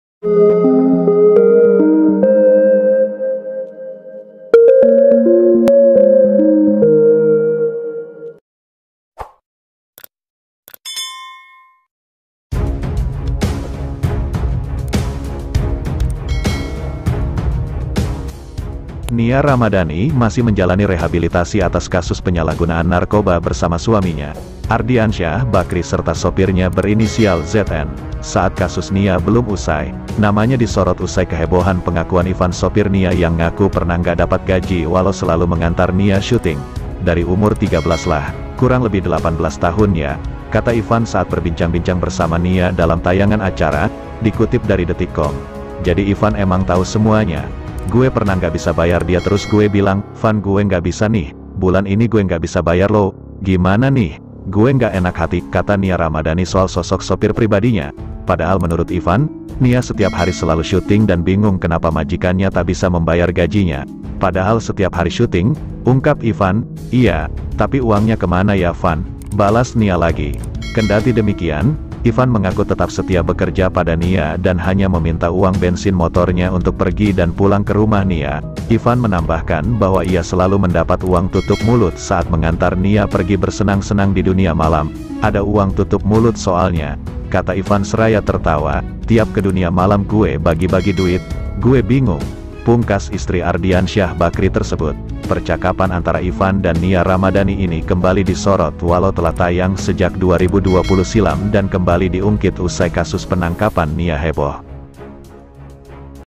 HEBOH Pengakuan Sopir Nia Ramadhani, Dapet Uang Tutup Mulut Tiap Antar Dugem⁉️
Berit4u Tv - Nia Ramadhani masih menjalani rehabilitasi atas kasus penyalahgunaan narkoba bersama suaminya, Ardiansyah Bakrie serta sopirnya berinisial ZN.

Saat kasus Nia belum usai, namanya disorot usai kehebohan pengakuan Ivan sopir Nia yang ngaku pernah nggak dapat gaji walau selalu mengantar Nia syuting.

"Dari umur 13 lah, kurang lebih 18 tahun ya," kata Ivan saat berbincang-bincang bersama Nia dalam tayangan acara, dikutip dari Detikcom.

"Jadi Ivan emang tau semuanya. Gue pernah nggak bisa bayar dia terus gue bilang 'Van gue nggak bisa nih. Bulan ini gue nggak bisa bayar lo. Gimana nih, gue nggak enak hati'," kata Nia Ramadhani soal sosok sopir pribadinya.

Padahal menurut Ivan, Nia setiap hari selalu syuting dan bingung kenapa majikannya tak bisa membayar gajinya.

Copy Right by Berit4u Tv